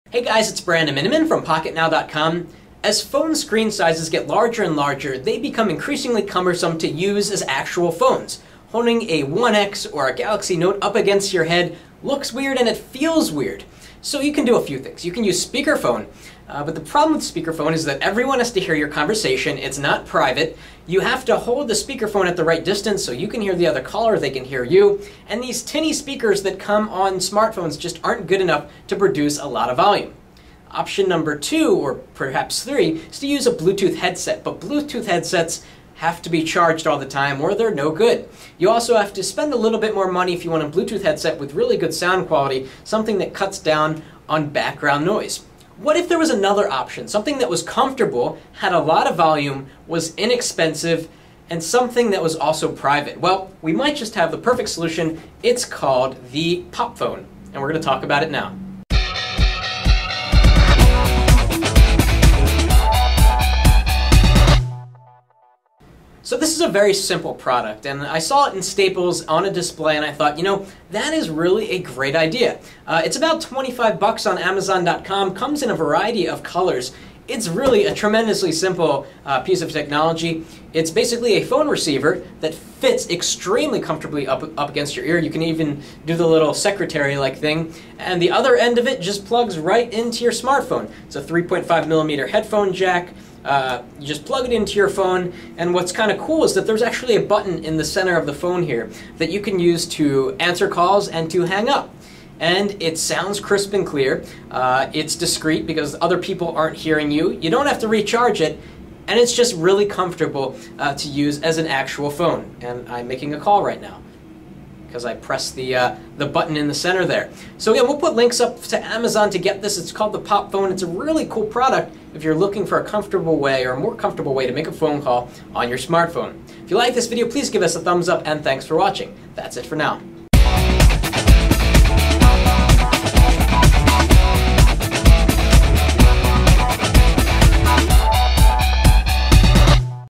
This Accessory Makes Talking on Your Smartphone Quite Comfortable | Pocketnow
Available from $17-25 on Amazon, the POP Phone is a simple product: it's a phone that plugs into your smartphone, giving you a comfortable talking experience. It uses a 3.5mm headphone jack, so it works with nearly any phone.